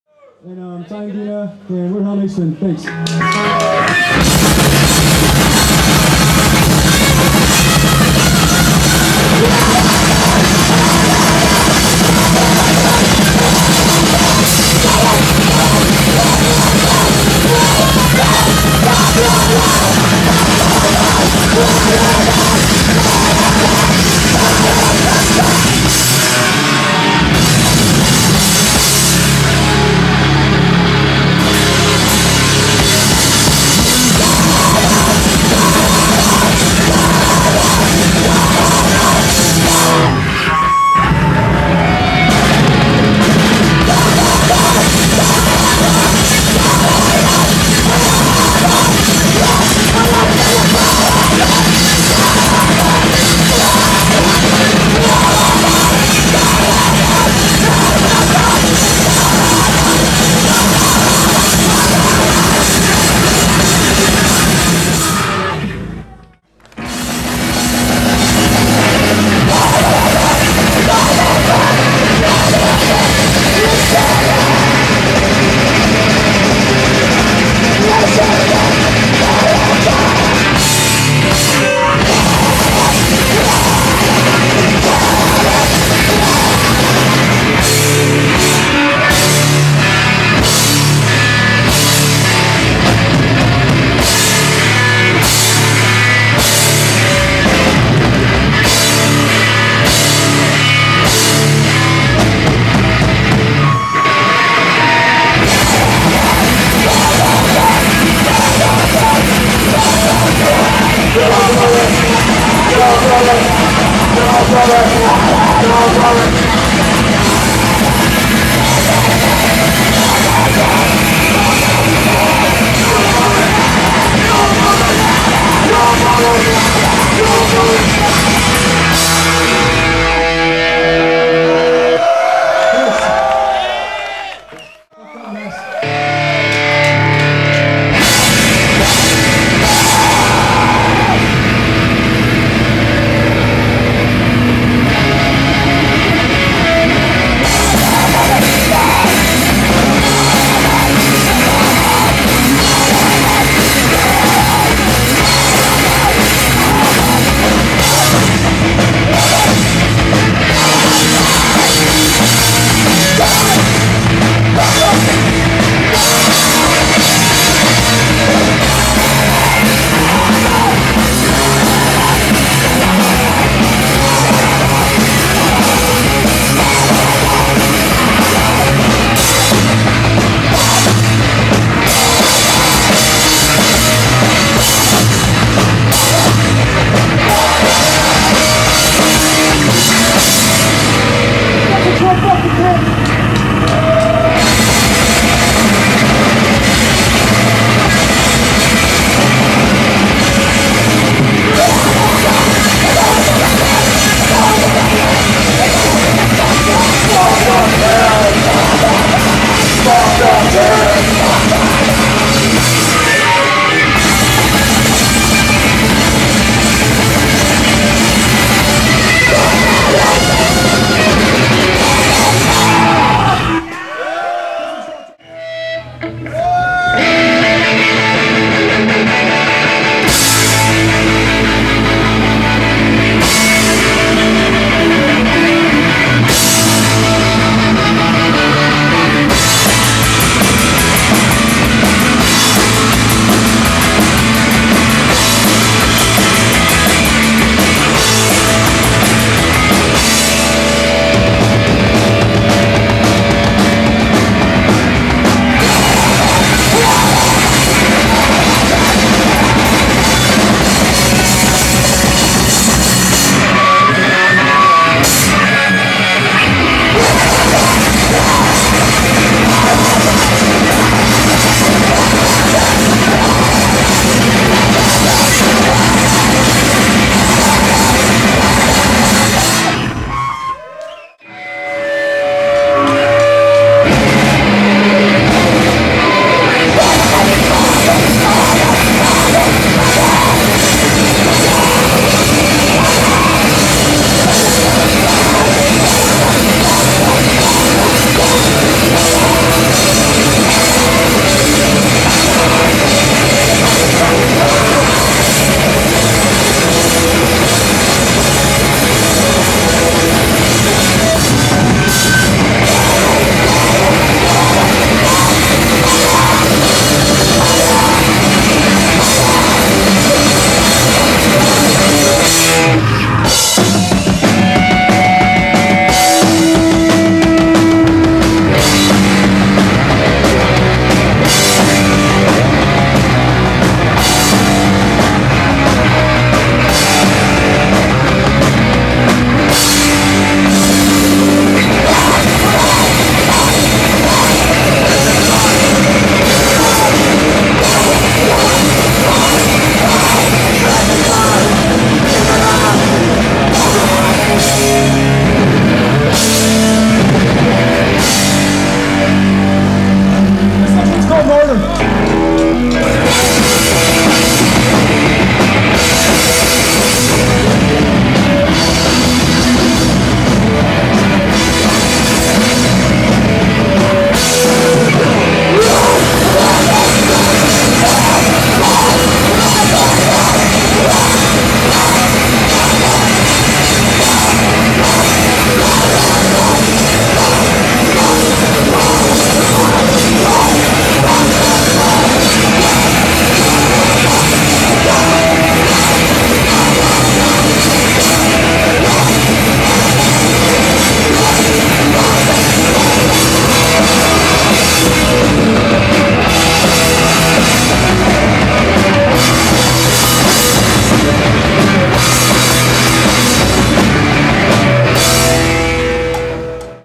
Hellnation live at Play Fast or don´t 2005
powerviolence band from Kentucky (USA) live in Czech.